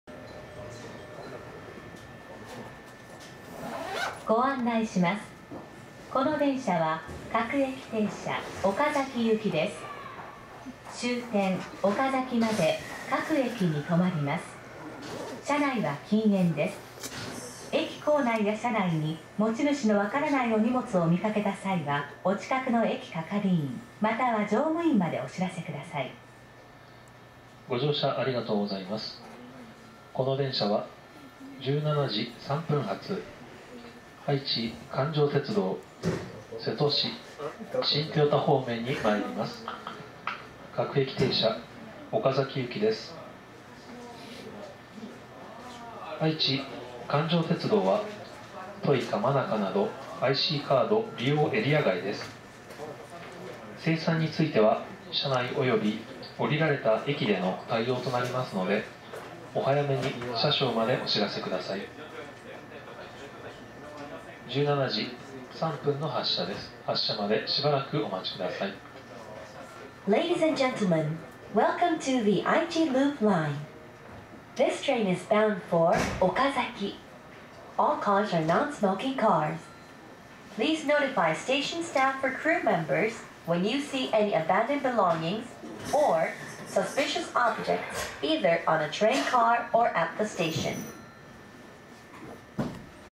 愛知環状鉄道岡崎行き案内放送
愛知環状鉄道の岡崎行き、高蔵寺駅停車中の案内放送です。